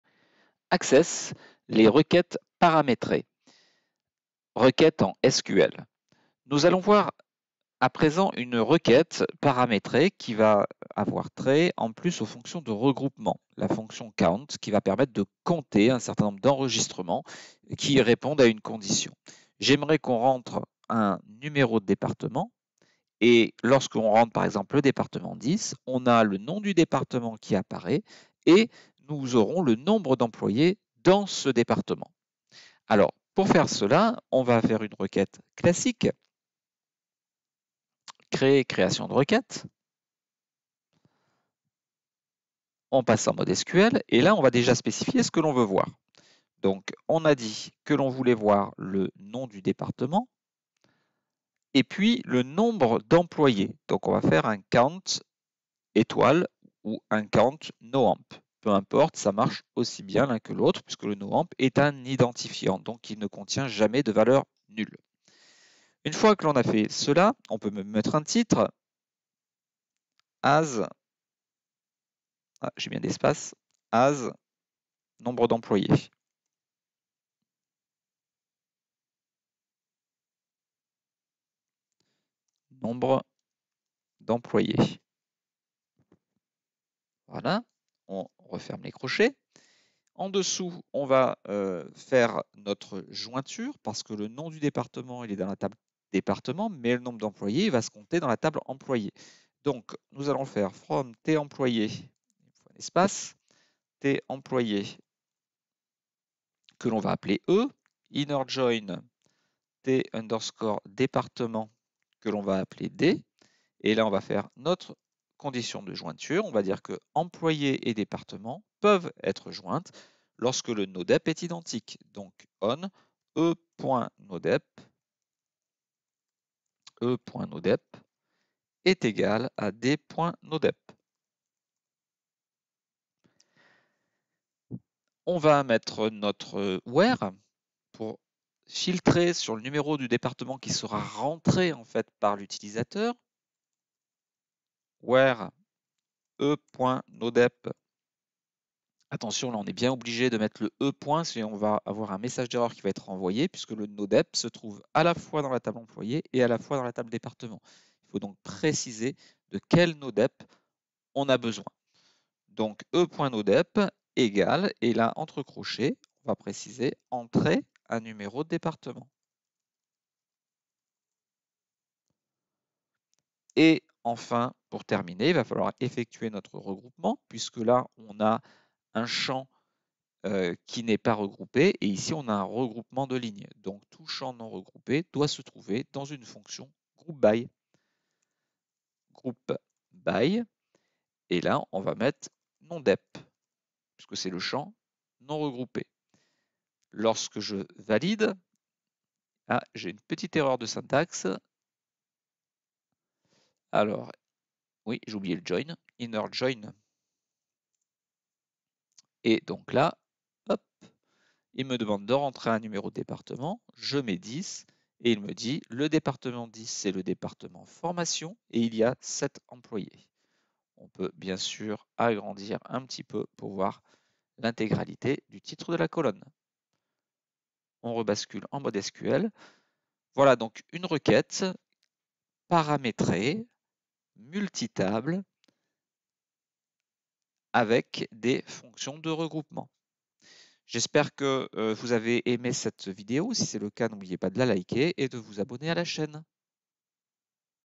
Requête Access SQL paramétrée 03
Concept étudié : Requête paramétrée multitable avec des fonctions de regroupement.
Exemple : Saisir un numéro de département et il apparait son nom et le nombre de salarié qui le compose.